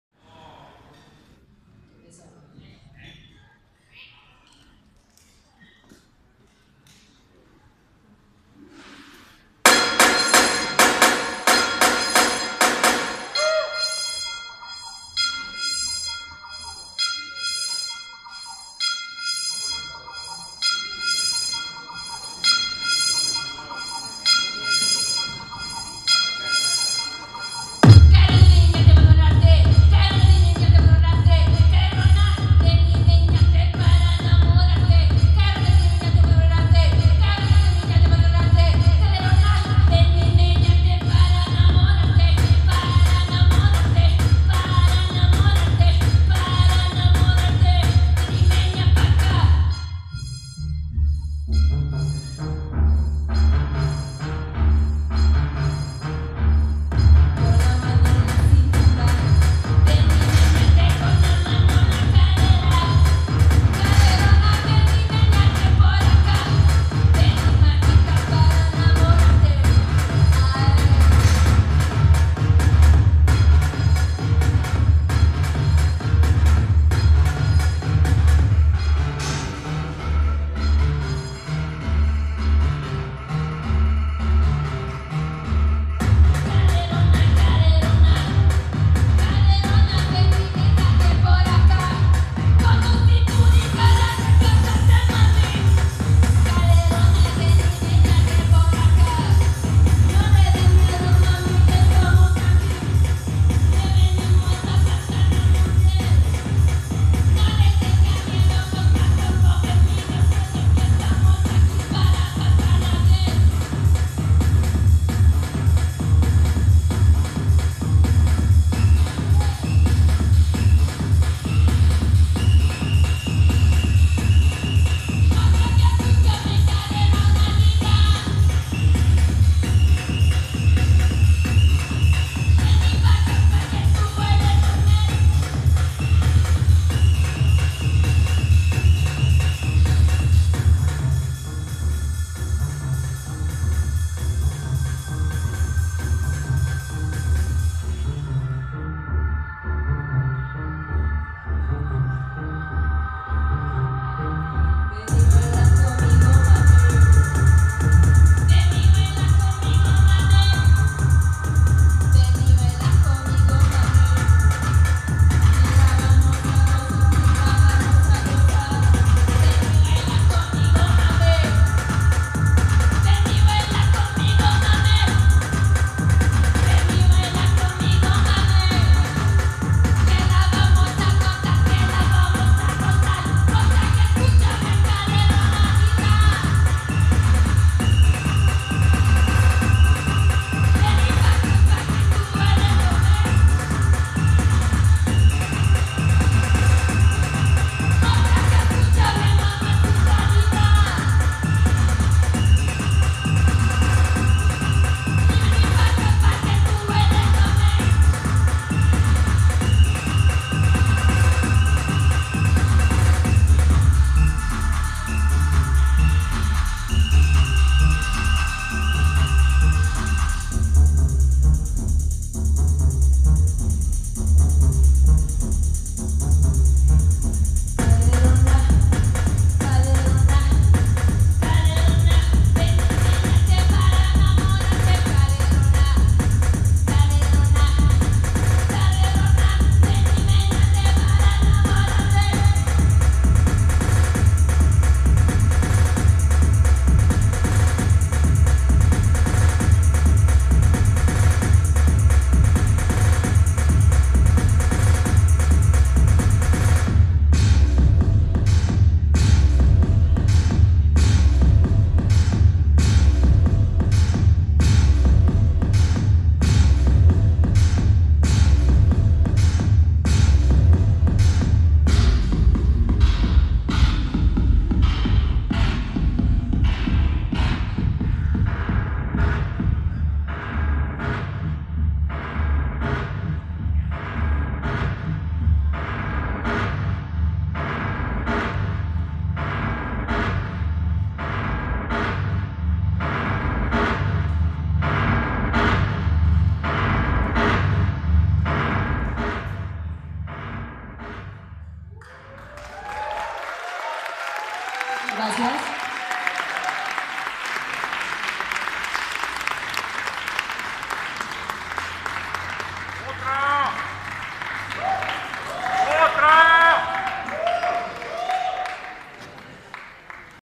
NODO Ciclo de conferencias.
Pelada en vivo en el Centro Cultural de España en México.